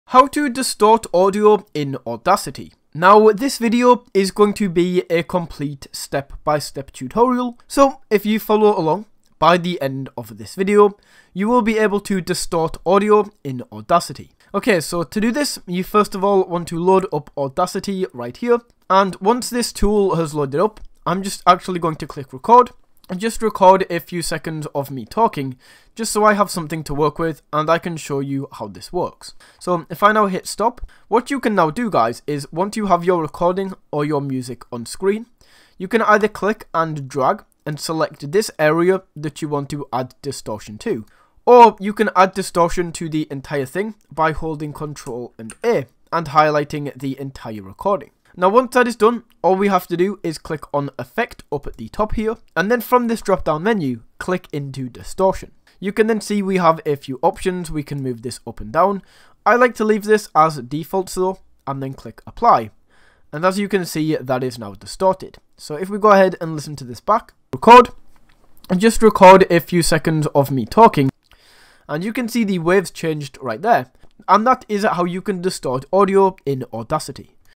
How To Distort Audio on Audacity
In this tutorial I will show you How To Distort Audio on Audacity